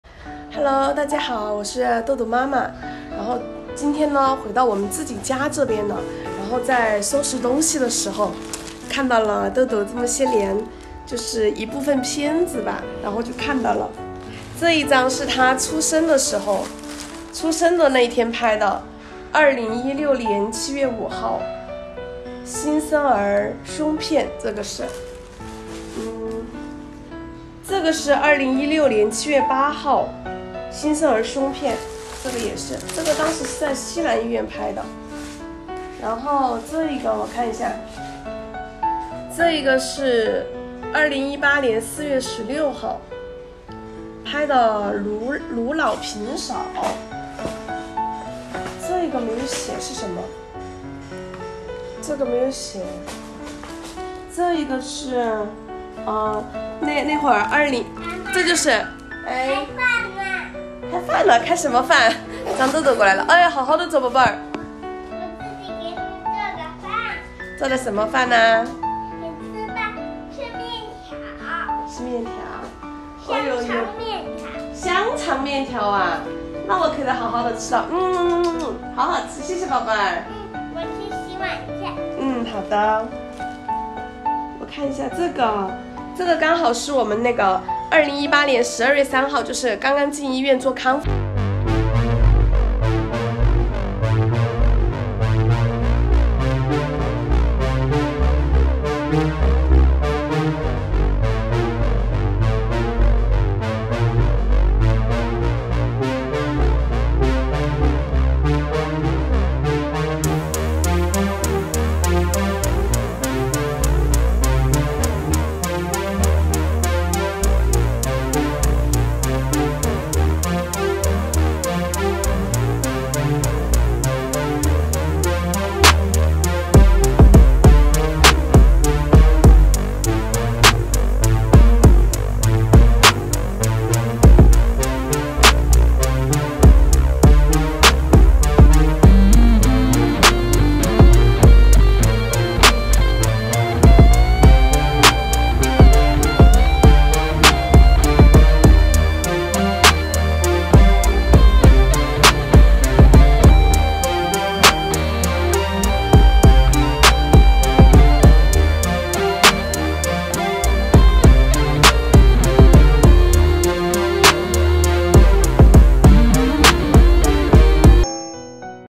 收拾屋子翻到了病历，豆豆此刻的出现让过往的心酸都画上了句号。
豆豆是2016年7月出生，早產缺氧缺血，醫學鑑定為腦癱兒
 一直在重慶進行康復治療，目前在學走路階段
在此謝謝大家對我們的支持和鼓勵，
疾病並不能阻止我們前進的腳步
即使跌倒一百次，也要站起來一百零一次
喜歡我的話關注我豆加油，感謝大家的支持： 请放心，本视频内容符合YouTube社区准则，没有任何违反规定的行为。